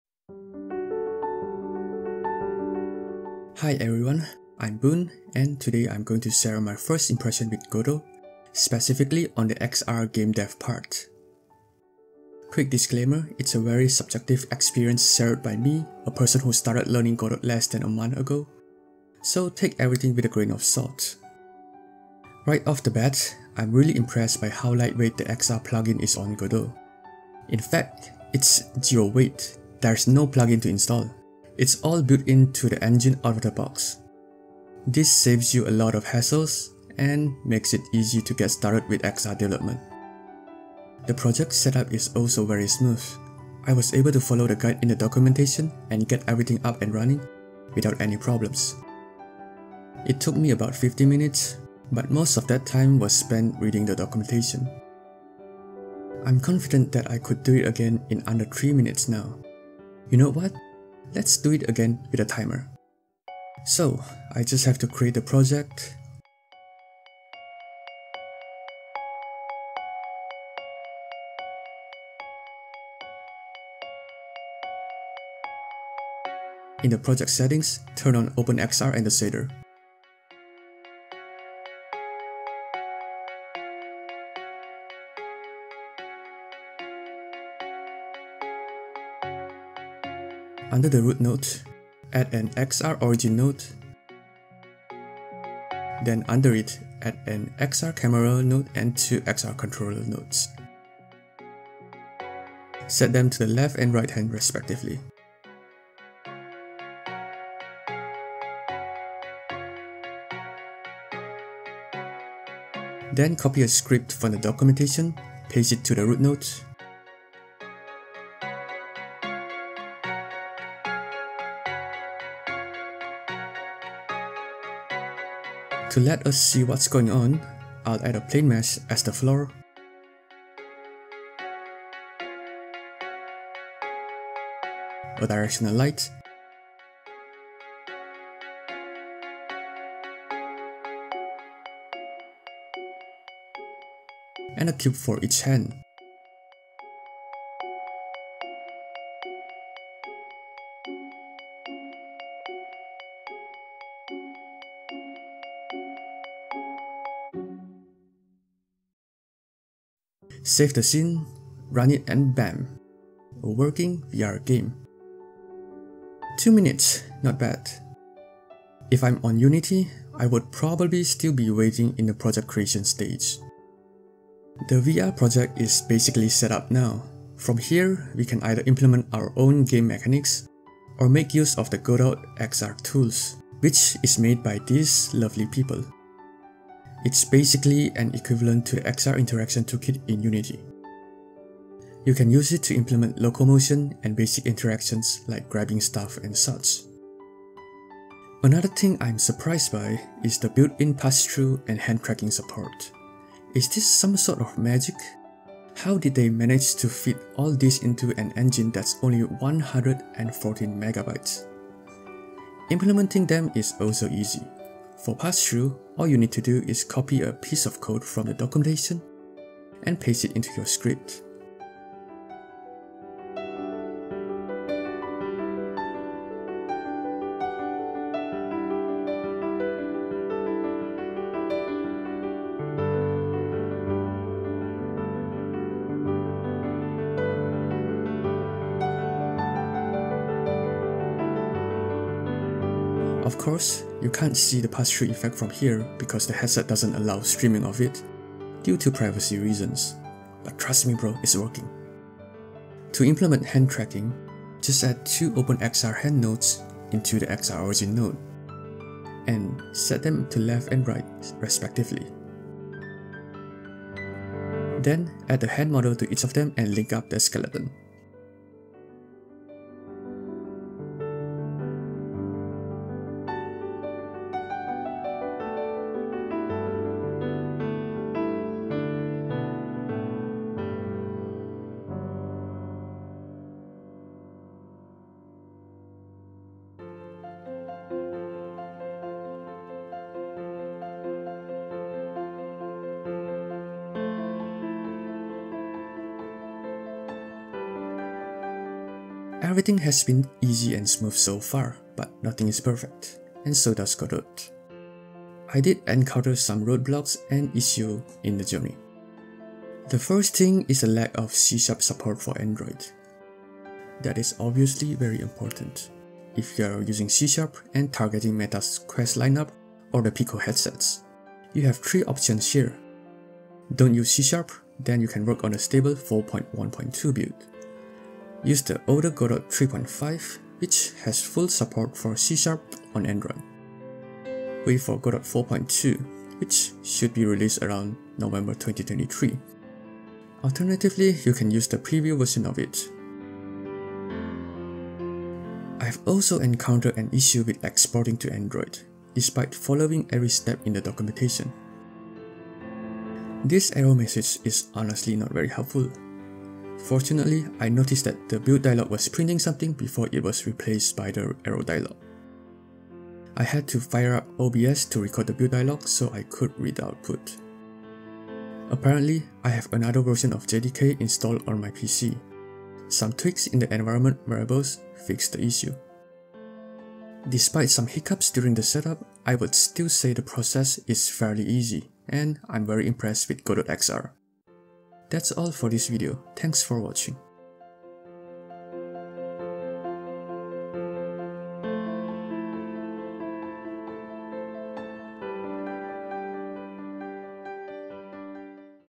Setting up XR/VR on Godot 4 | First Impressions, Passthrough, Hand tracking
In this video I shared my first impressions on the XR setup on Godot 4.

Tempted to make a tutorial out of it, but the guide on the documentation covers the setup process pretty well. Also, I guess speed-running it is more fun.

I was trying to search for the "Oculus Integration" plugin (because that's needed on Unity), and was genuinely surprised by the fact that passthrough and hand-tracking features came with Godot out of the box.

Overall, very easy to setup, and I would definitely recommend it to anyone who is interested in developing VR games.